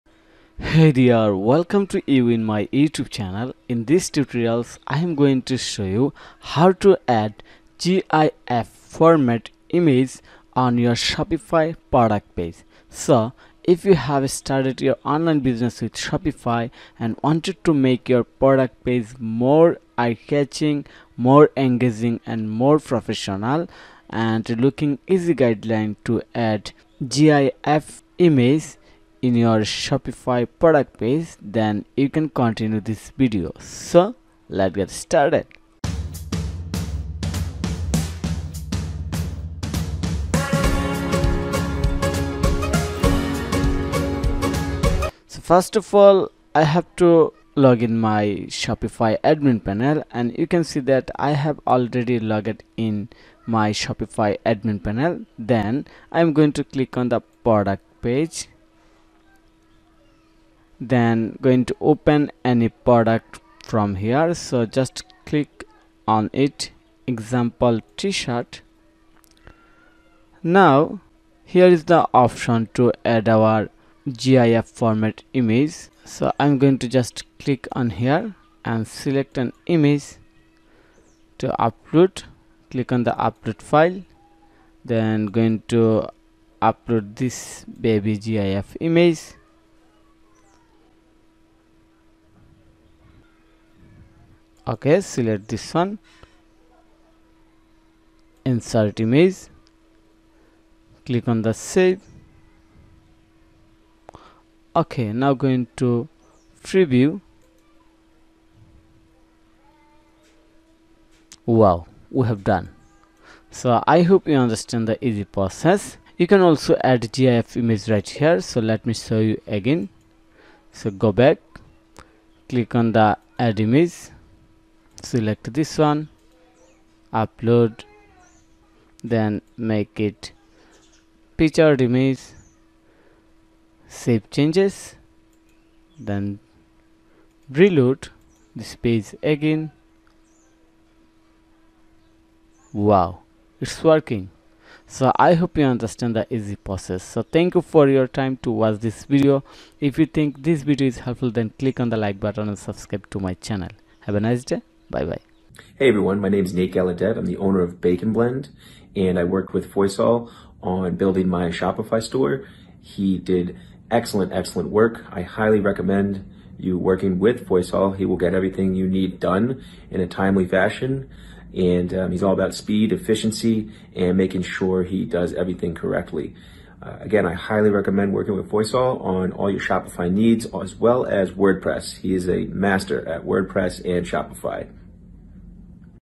How to add GIF Image on Shopify Product Page Description in 2025 ✅ Shopify Tutorial
# GIF

💻 Welcome to * How to add GIF Image on Shopify Product Page Description in 2025 ✅ Shopify Tutorial *

📝 Video Description: In this Shopify tutorial, learn how to add GIF images to your Shopify product page descriptions in 2025! GIFs can make your product pages more engaging, fun, and informative, helping you convert more visitors into customers. We’ll walk you through a step-by-step process to upload and embed GIFs seamlessly into your Shopify product descriptions, even if you're not tech-savvy. Enhance your store's user experience with this quick and easy method. ✅

Benefits of Adding GIFs to Your Shopify Product Pages:

Increased Engagement: GIFs grab attention and make your product pages stand out.

Improved Communication: Use GIFs to demonstrate product features or show the product in action.

Enhanced Conversion Rates: A lively product page with GIFs can boost customer trust and increase sales.

SEO Boost: GIFs can help reduce bounce rates, signaling better engagement to search engines.
Memorable Branding: GIFs add a creative touch to your store, leaving a lasting impression.